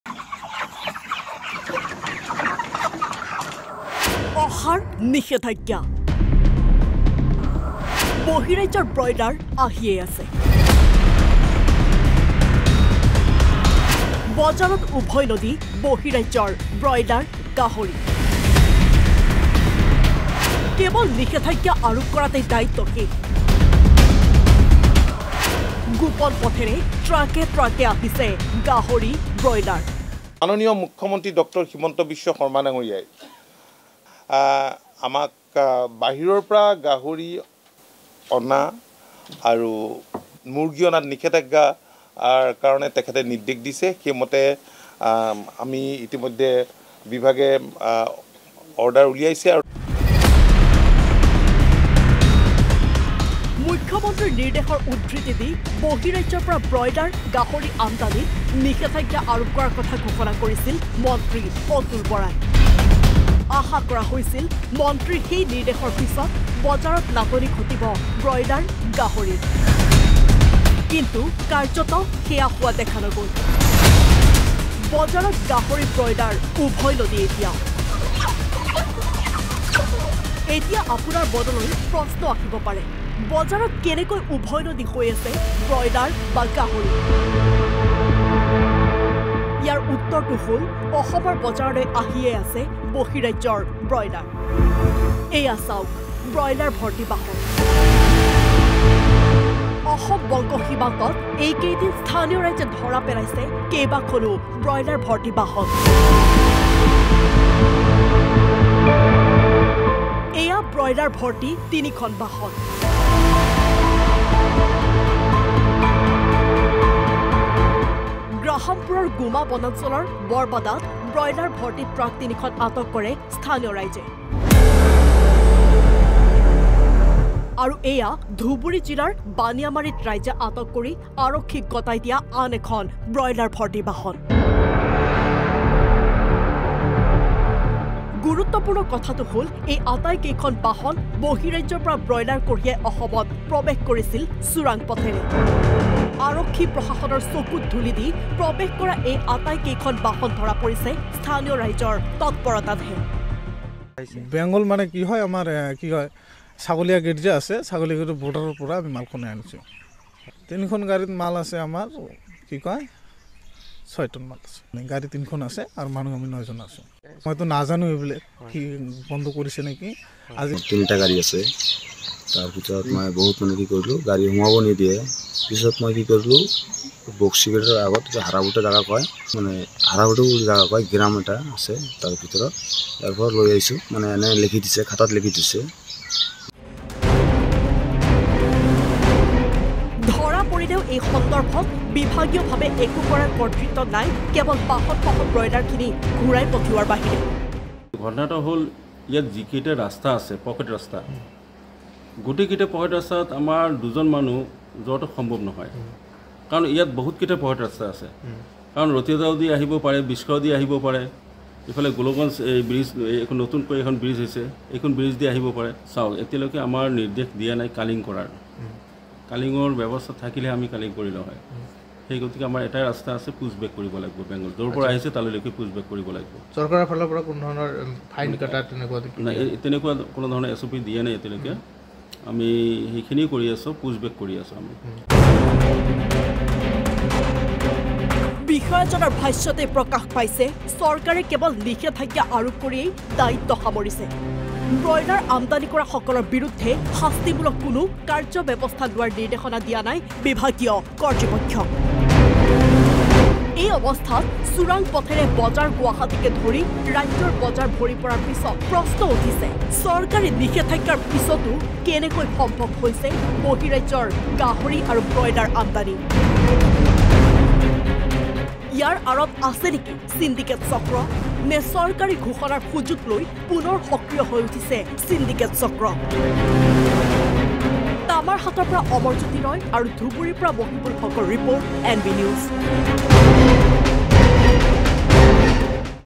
অসাৰ নিষেধাজ্ঞা!, বহিঃৰাজ্যৰ পৰা আহিয়েই আছে ব্ৰইলাৰ।
অসাৰ নিষেধাজ্ঞা!, বহিঃৰাজ্যৰ পৰা আহিয়েই আছে ব্ৰইলাৰ। গোপন পথেৰে ট্ৰাকে ট্ৰাকে আহিছে গাহৰি-ব্ৰইলাৰ